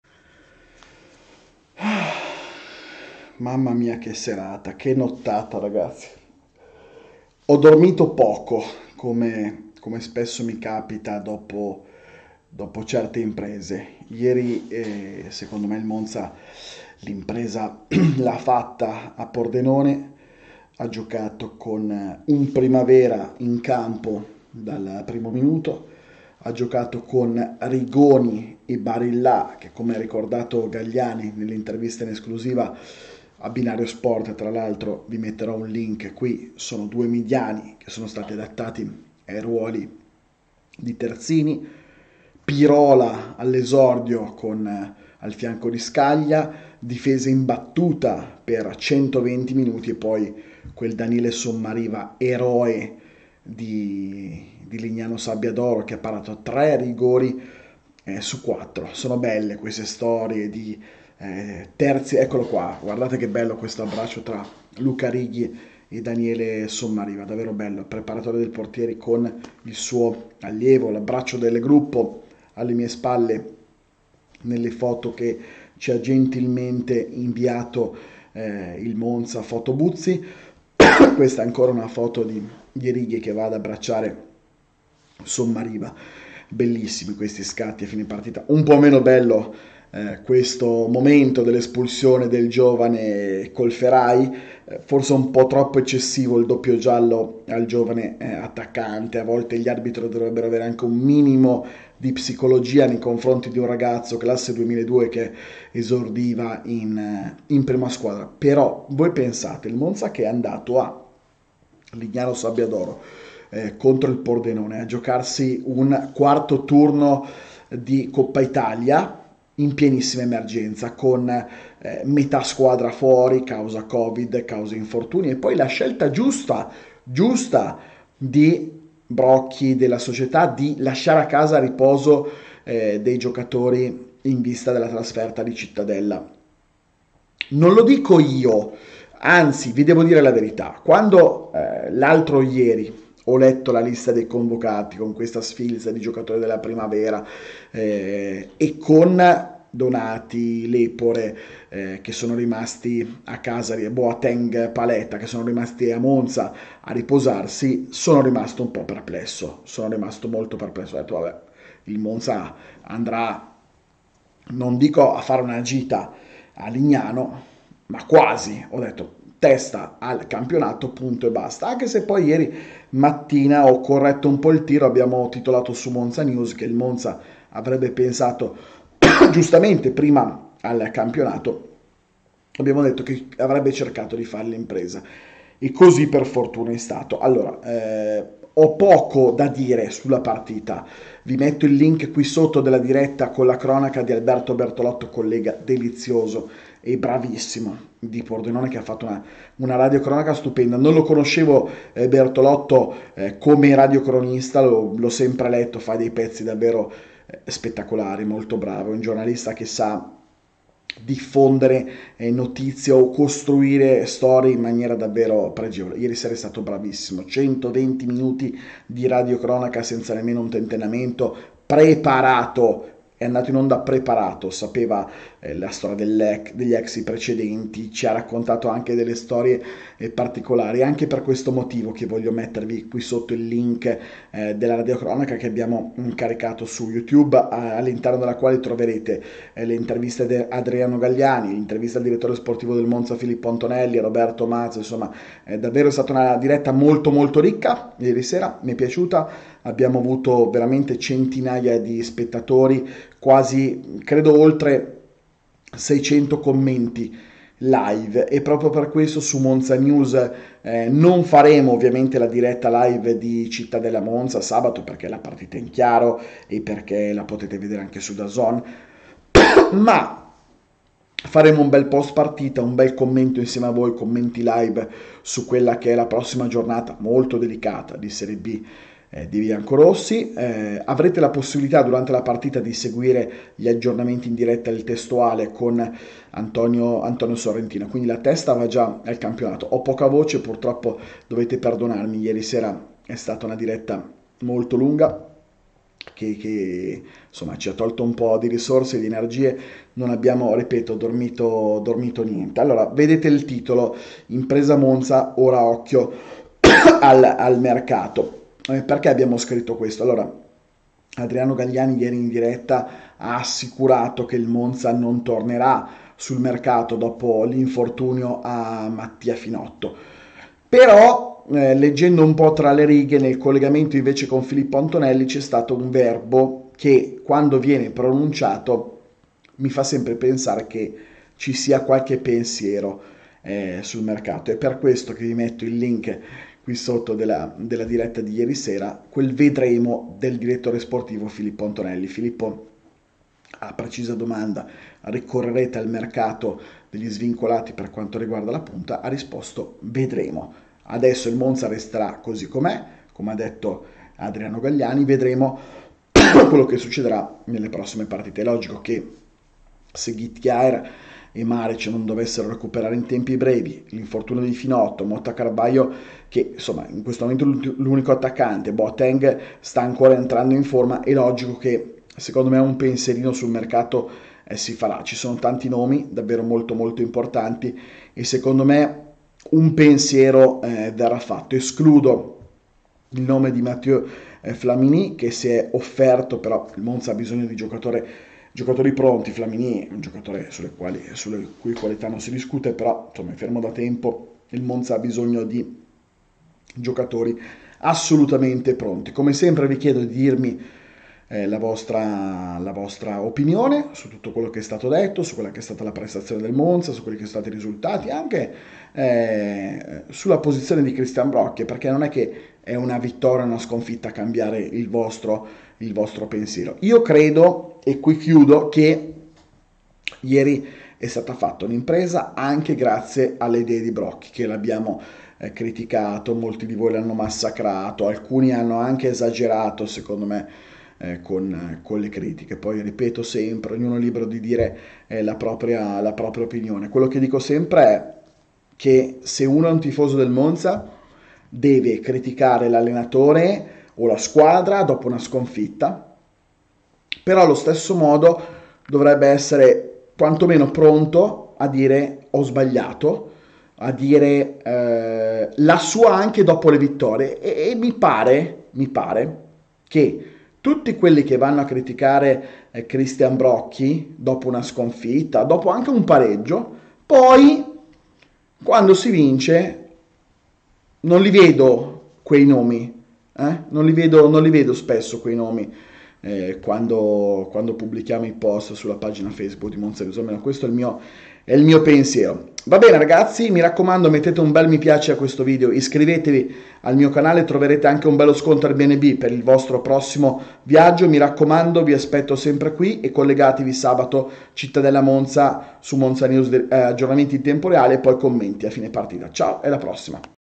Coppa Italia Pordenone - Monza 1-4 (d.c.r.) | Il Monza fa l'impresa in Coppa, arriva un attaccante?
In piena emergenza, il Monza fa l'impresa e passa il 3° turno di Coppa Italia. Arriverà un attaccante svincolato? Il punto di Stefano Peduzzi.